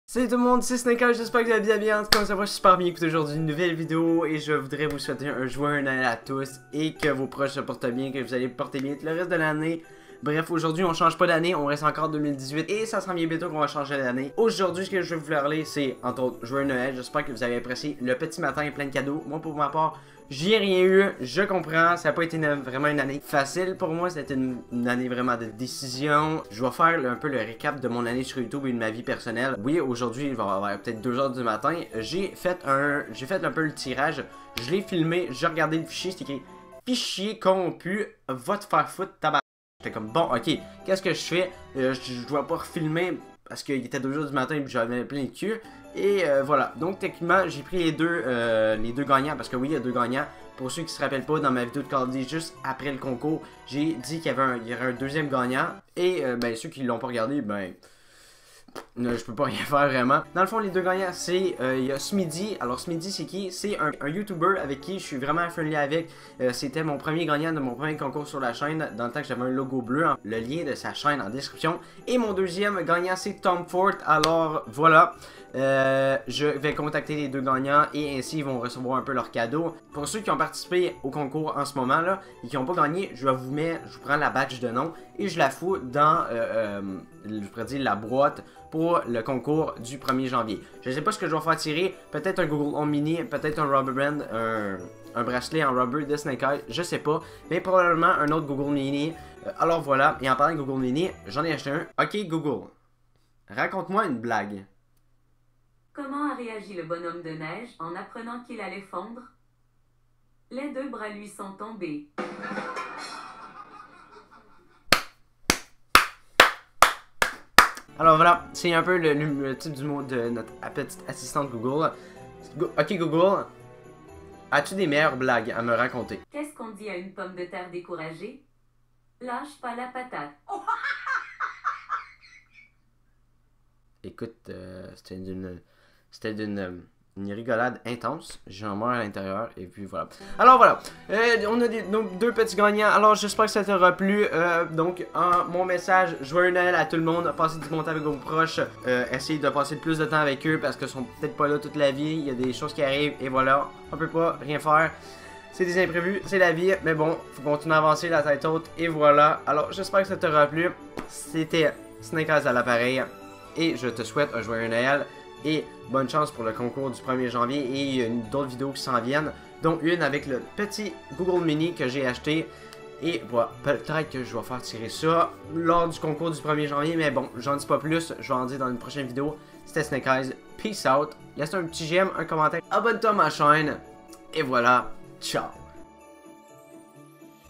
Joyeux Noël 2018 ! *Résultat Concours*
Joyeux Noël 2018 ! J'espère que vous avez eu plein de cadeaux ! Je vous parle du résultat du concours et aussi de quelque autre petites informations rigolote.

► LE MEILLEUR DU GAME ! GamerCrates

Vidéo uploadé & crée appartenant à/par Snakeyes™

& PEACE OUT POUR UNE PROCHAINE VIDÉO !
A+